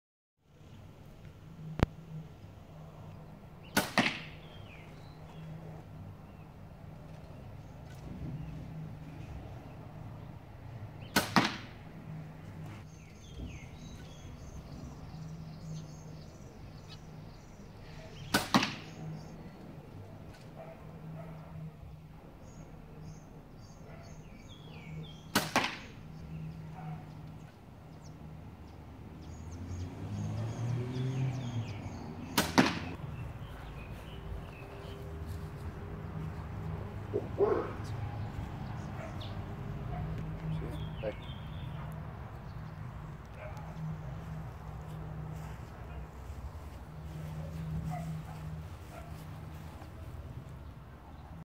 Hoyt CRX 35 Pro Series Compound Bow
Testing the Hoyt CRX 35 pro series. Close range, 15 Metres. (HD).